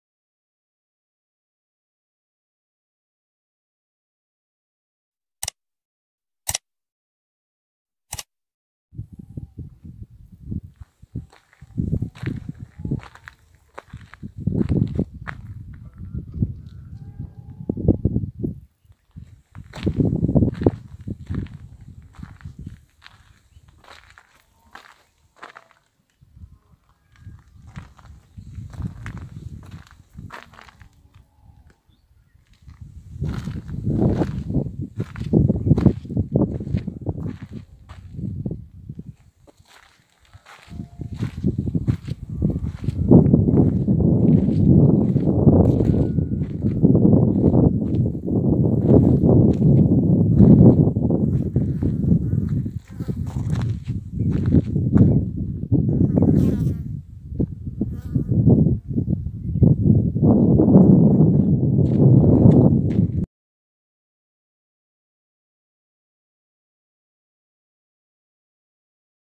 ناقتين حليب وهداوه لقحات تلد شهر ثلاثه للبيع التفاصيل☜( تم البيع )
حراج على  ناقتين لقحات تلد شهر ثلاثه
سالمه جميع العذاريب وهدايا
السن / كبار
الموقع /  وادي ترج بيشه